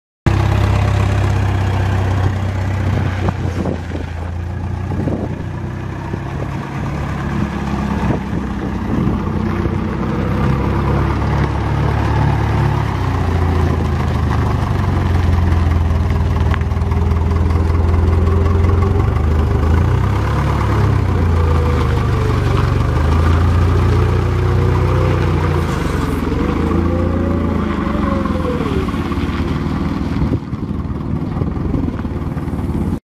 HYSTER H60F For Sale
Hyster H60F forklift
selling for local construction company
runs and drives and operates
shuttle shift with one foot
single handle control
Buy upon your inspection
Pick items up promptly after sale
Located at the Kobza Auction Building in David City